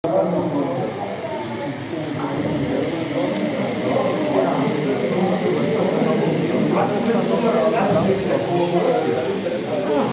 BR 96 SNCB (3101) mit SNCB-Personenwagen
Die Ex T-12 (3095) in belgischer Lackierung mit einem belgischen Donnerbüchsen Nahverkehrszug. Passende Kombination.